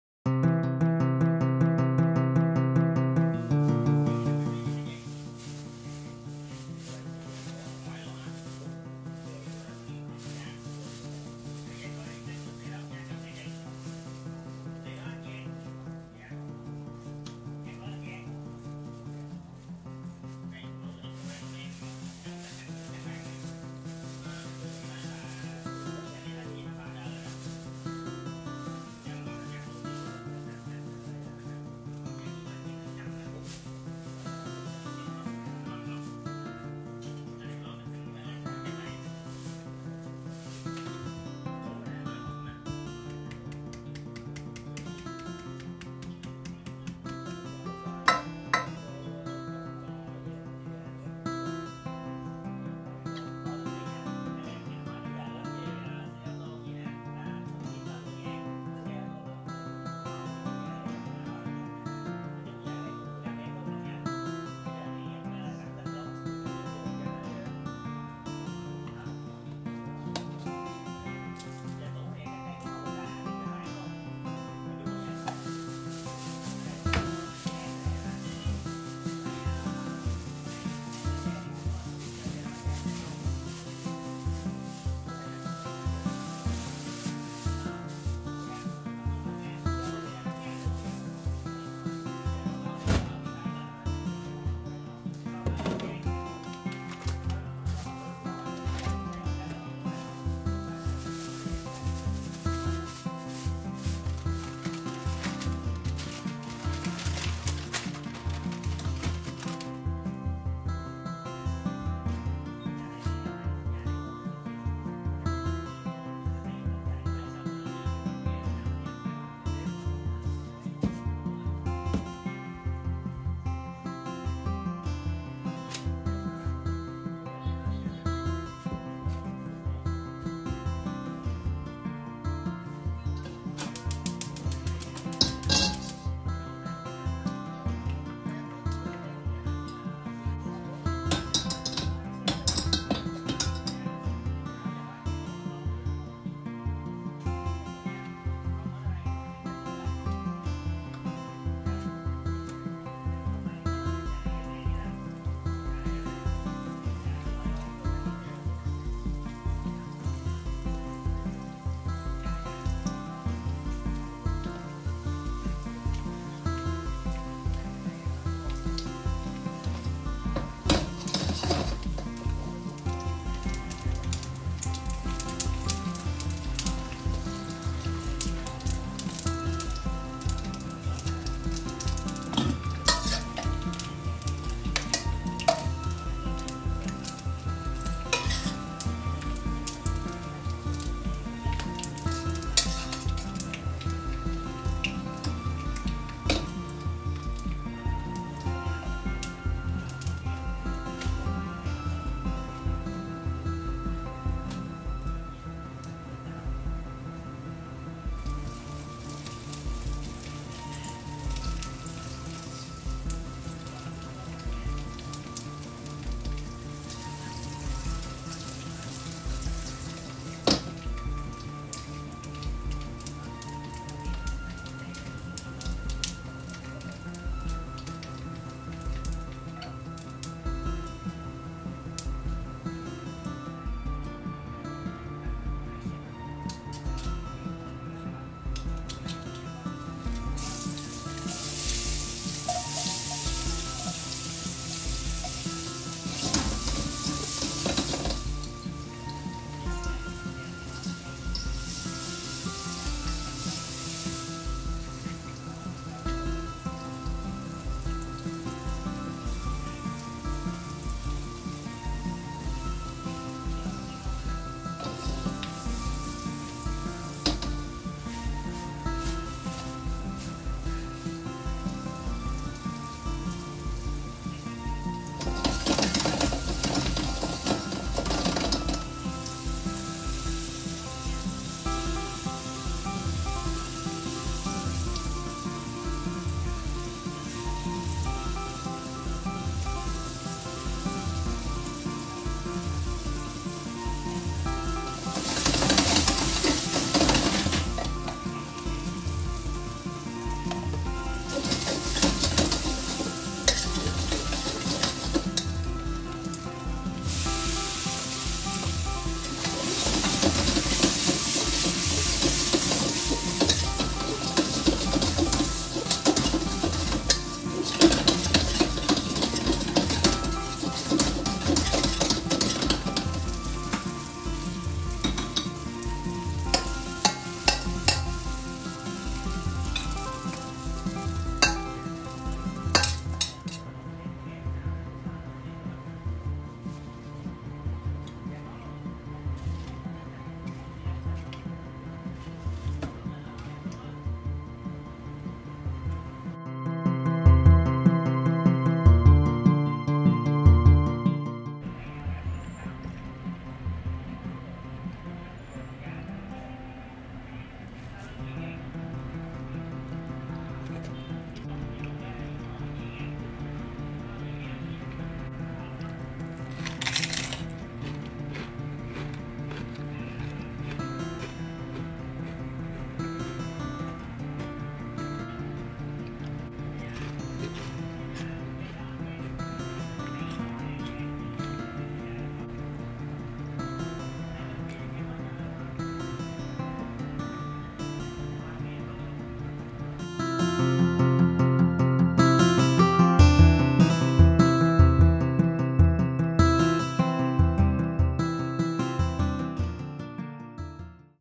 Salmon Skin Chips & Fried Beehoon
I made tasty chips and beehoon with the salmon skins this time.

And also…